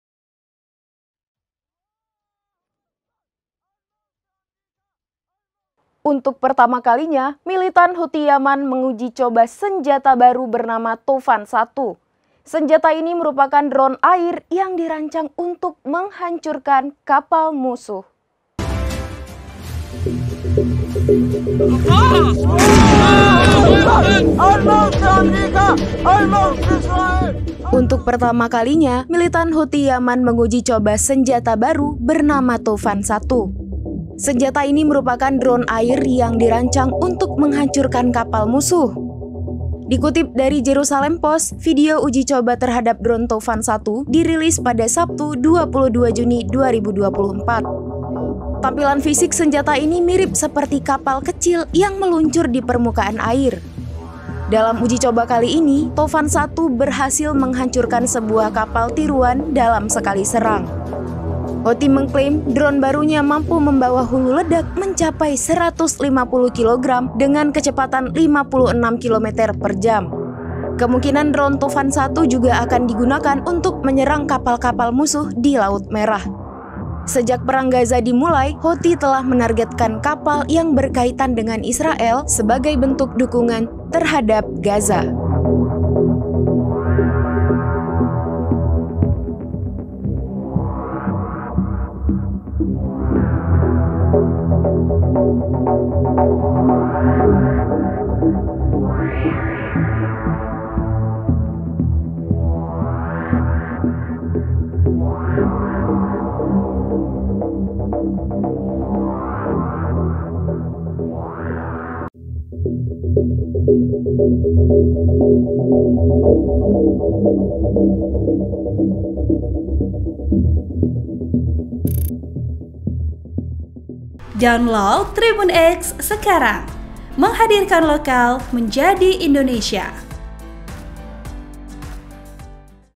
Canggih! 'Toufan 1' Houthi Yaman Sekali Serang Kapal Musuh Hancur
SERAMBINEWS.COM - Untuk pertama kalinya, kelompok Houthi Yaman menguji coba senjata baru bernama Toufan-1.

Artileri mematikan ini merupakan drone air yang yang dirancang untuk menghancurkan kapal musuh.

Dikutip dari Jerusalem Post, video uji coba terhadap drone Toufan-1 dirilis pada Sabtu (22/6/2024).

Tampilan fisik senjata ini mirip seperti kapal kecil yang meluncur di permukaan air.

Dalam uji coba kali ini, Toufan-1 berhasil menghancurkan sebuah kapal tiruan dalam sekali serang.

Houthi mengklaim, drone barunya mampu membawa hulu ledak mencapai 150 kilogram dengan kecepatan 56 kilometer per jam.

Kemungkinan drone Toufan-1 juga akan digunakan untuk menyerang kapal-kapal musuh di Laut Merah.

Sejak perang Gaza dimulai, Houthi telah menargetkan kapal yang berkaitan dengan Israel sebagai bentuk dukungan terhadap Gaza.(*)

VO: Syita
Editor: Raja